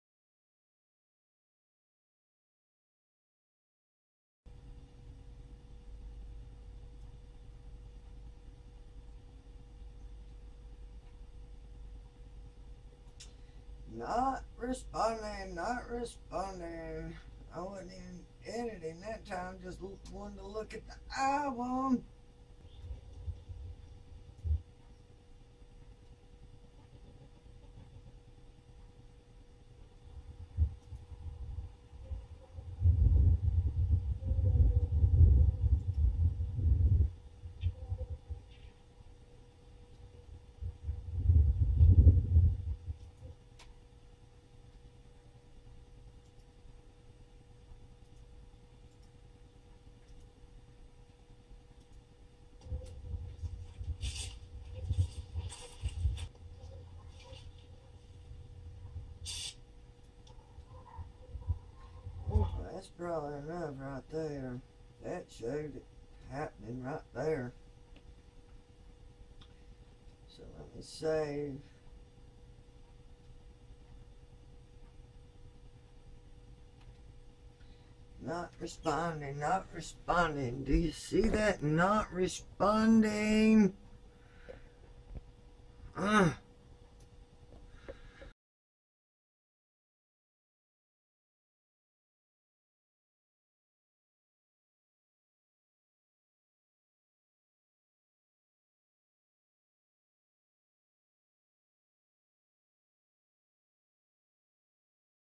Wondershare Video Editor (Not Responding) Very Frustrating!
This is so frustrating. Wondershare gave me instructions to turn off any anti virus and firewall and to download again. And to send them screen shots of any error mesages which I didn't have while downloading and to send log files. I did everythng they told me to do, It is still not working well at all. They have not responded to even let me know if they are working on it. I don't know what to do. So I suffered through all the stalling to put up this video. Now I will e mail them with a link to this video and it is up for the whole world to see. Thanks to Wondershare.
It is Feb. 2015 and wondershare still is not able to resolve this problem.
 If anyone can tell me a really good Editing software that I can buy if wondershare can't fix this problem and if they give me a refund I sure would be glad to hear it..
Feb. 26. 2015  They could not fix the problem with their software so I have uninstalled and asked for my money back. Still waiting.